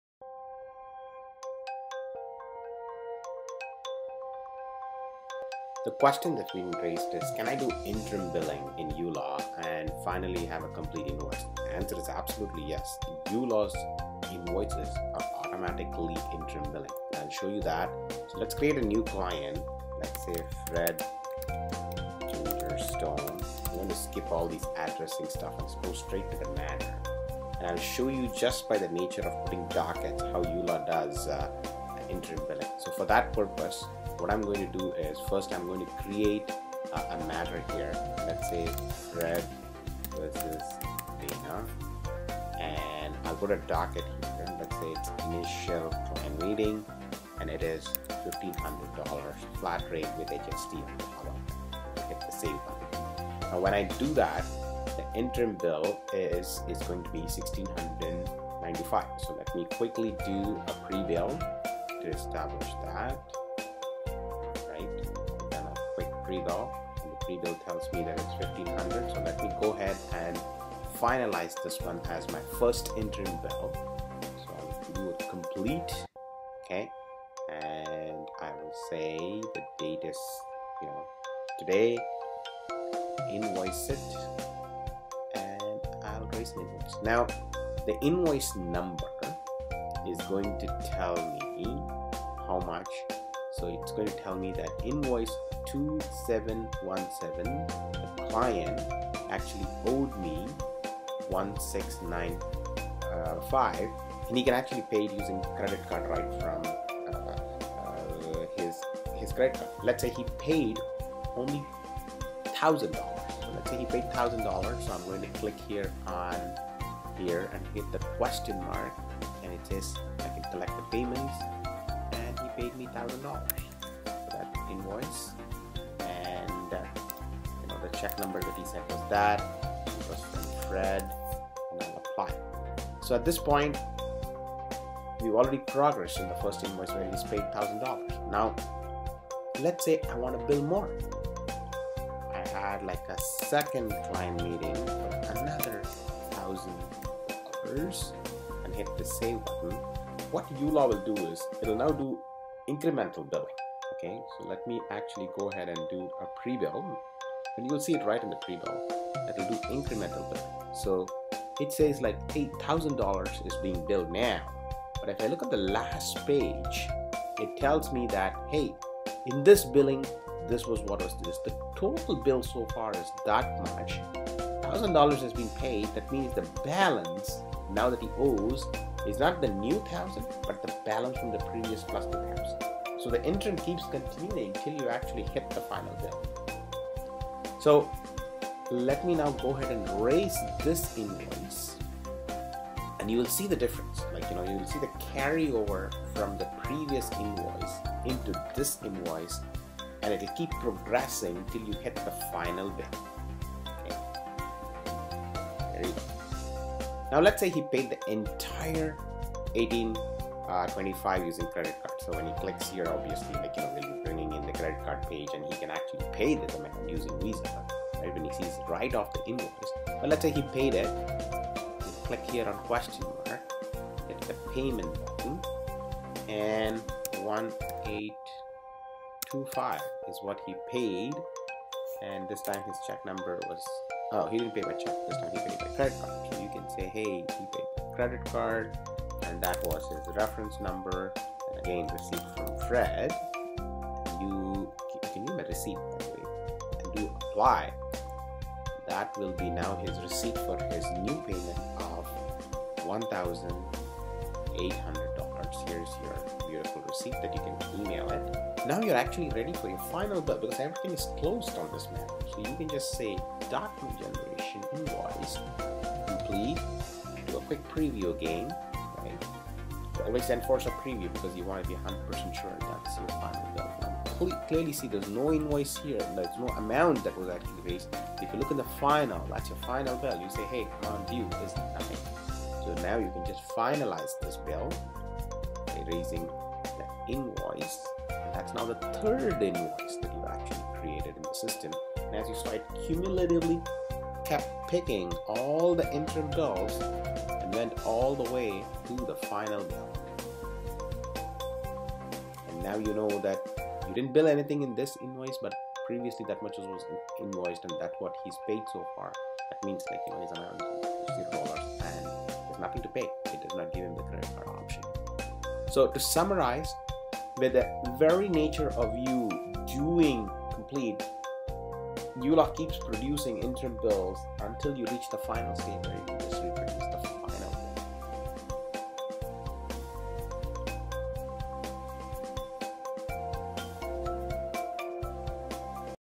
Q181. Does do I manage my Interim Bill and Final Bill in uLaw?
uLaw supports what we call iterative billing which means it can take all billing and balance thus far in any interim bill into the next bill Thus making it easy for you to manage your client billing. Keep it simple and keep billing.